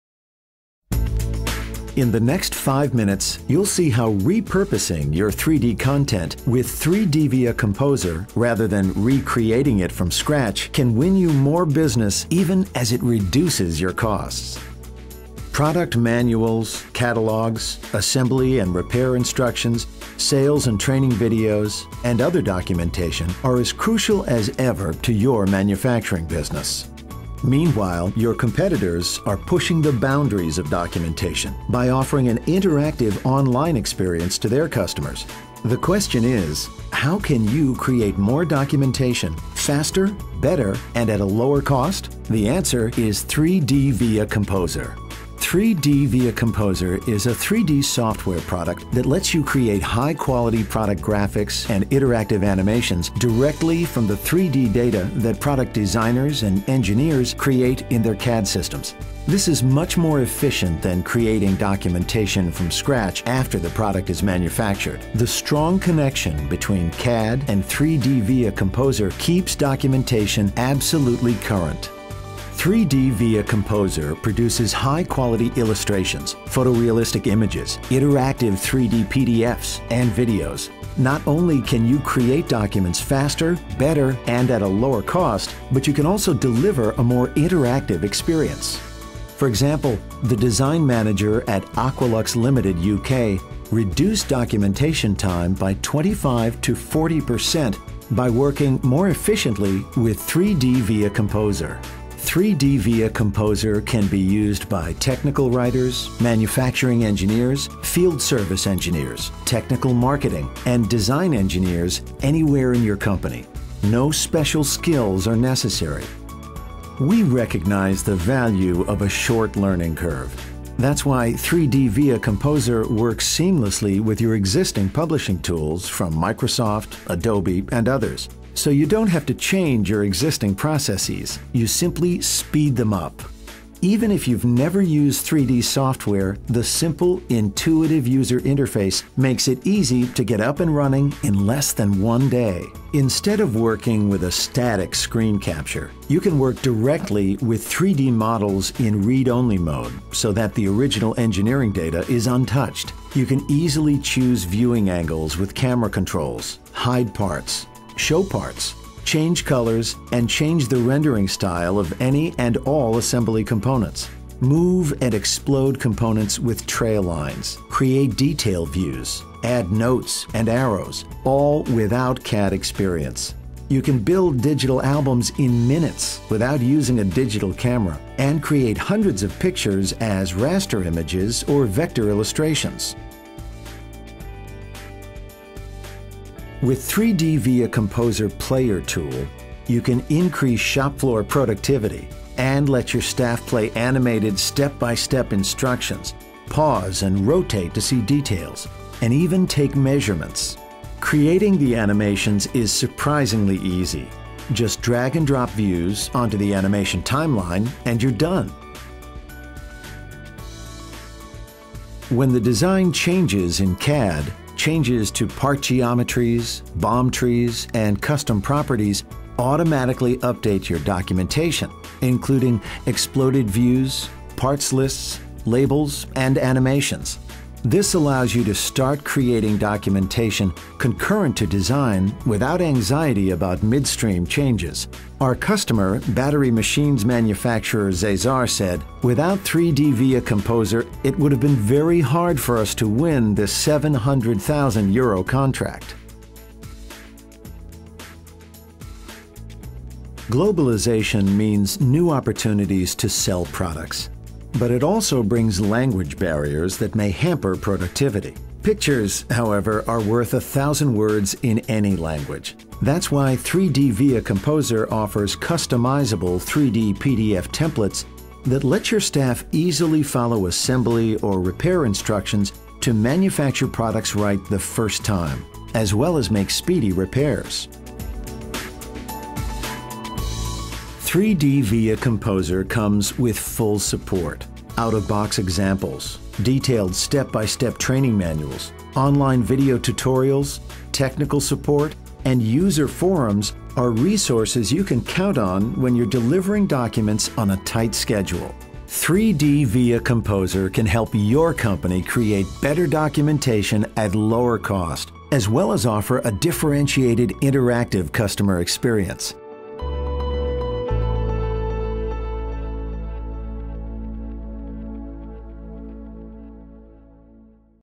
SolidWorks and 3DVIA Composer
Have your products ever been delayed because the documentation wasn't ready? Or, have you ever lost out on a bid because your presentation materials weren't compelling enough? You're not alone.

Instead of waiting until a project is done to get started, 3DVIA Composer lets you start creating documents almost as soon as the design process begins. See how SolidWorks and 3DVIA Composer work together to help you be more productive and win more business.